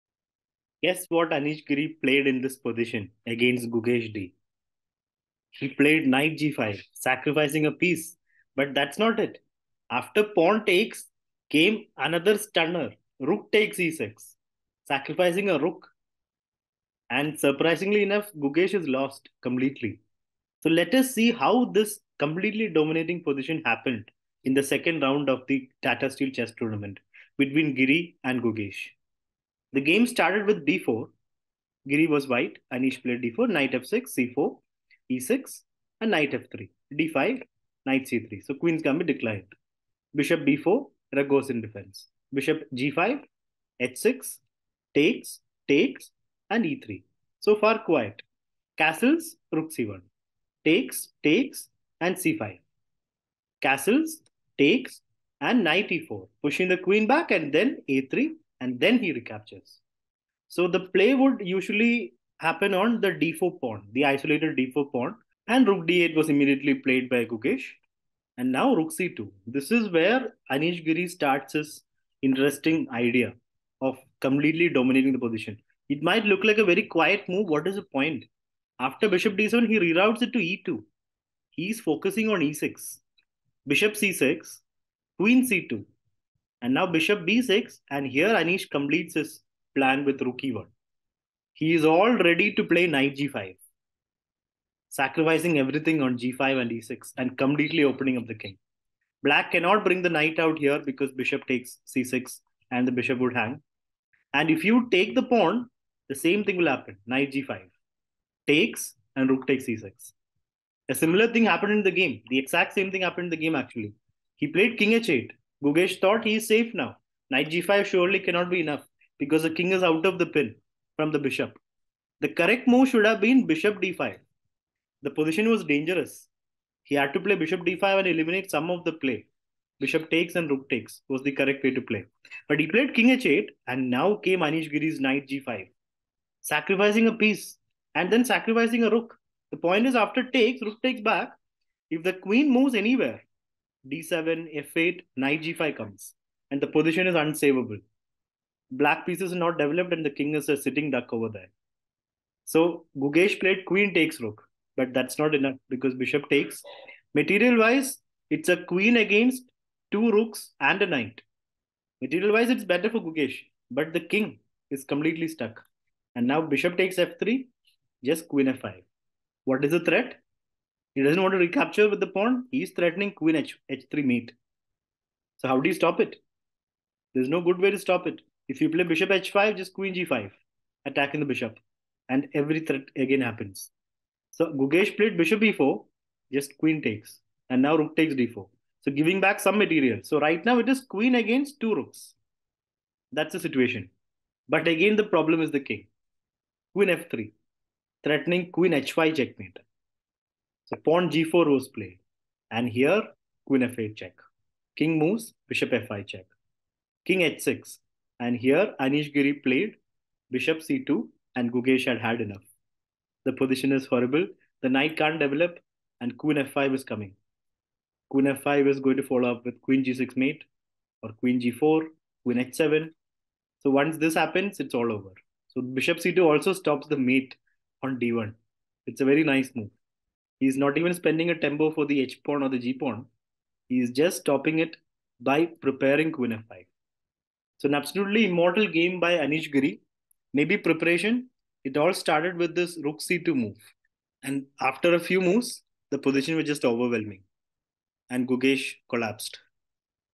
Anish Giri's Immortal | Tata Steel Chess Tournament 2023
Anish Giri stunned Gukesh with an amazing knight sacrifice and then a rook sacrifice to follow it up. This was like watching a musical melody unravel.